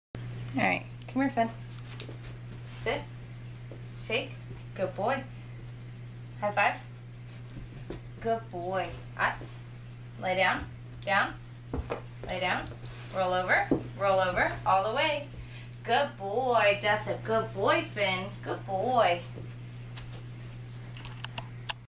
Video of adoptable pet named Poe
Poe is an adoptable pet with Ewenity Farm Border Collie Haven, Inc. in Bradenton Fl.